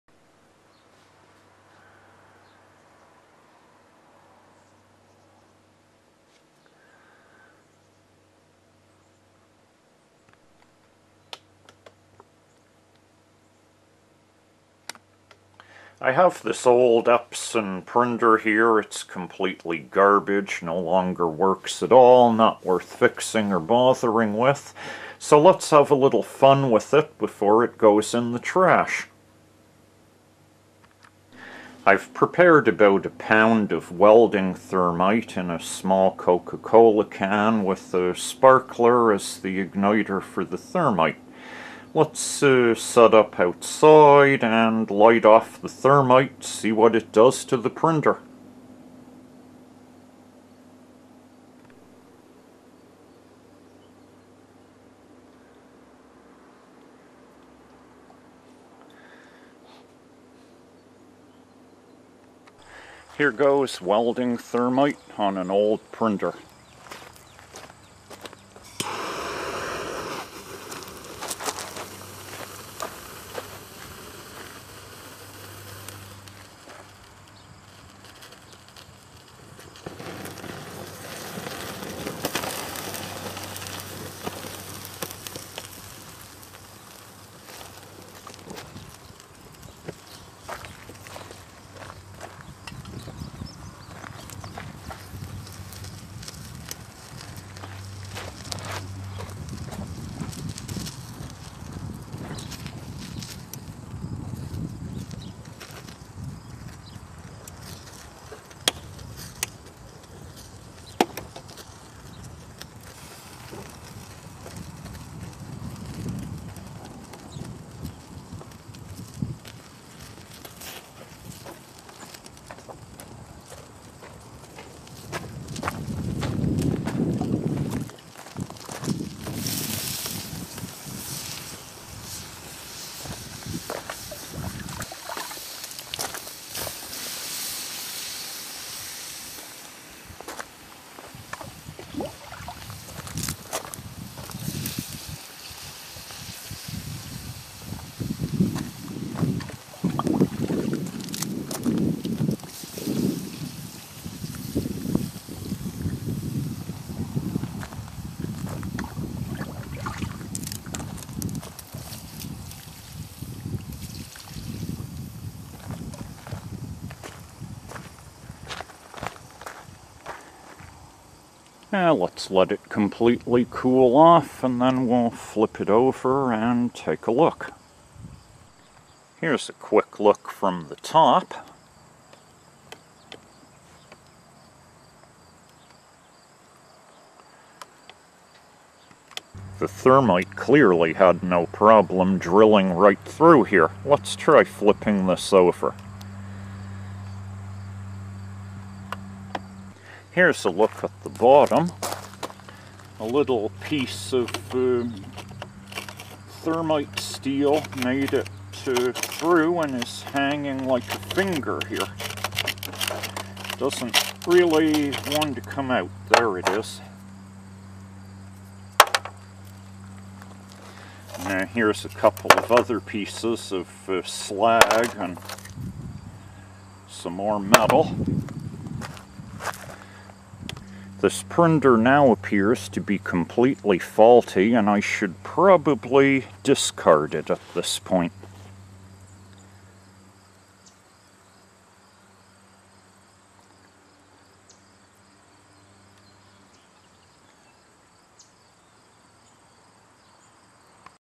Thermite On An Old Printer
I had a dead printer to be discarded so why not have some fun with it first. About a pound of welding thermite was placed in a small cola can. A sparkler was provided as an ignitor. The thermite had no problem melting right through.